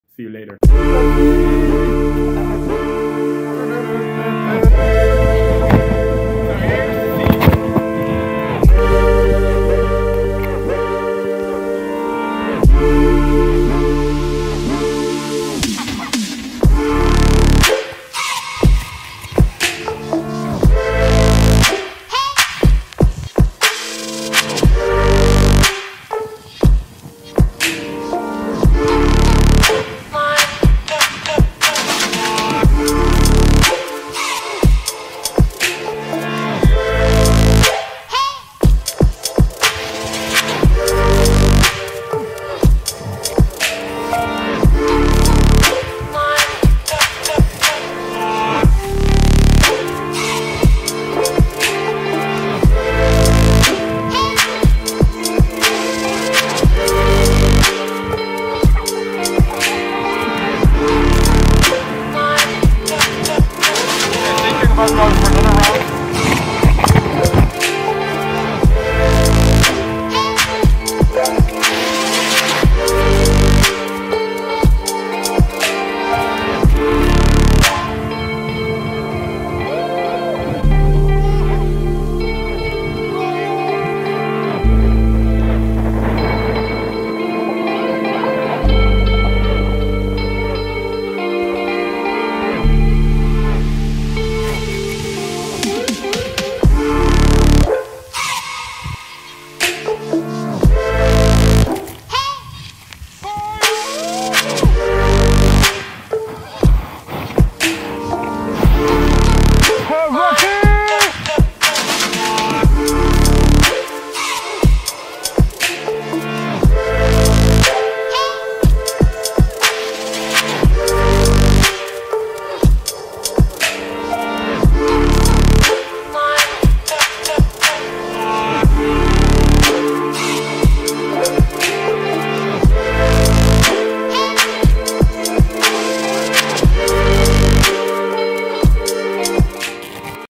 Contraloop kitesurfing day - Egmond aan Zee - The Netherlands
Not the best loops but we had fun at Egmond aan Zee the Netherlands!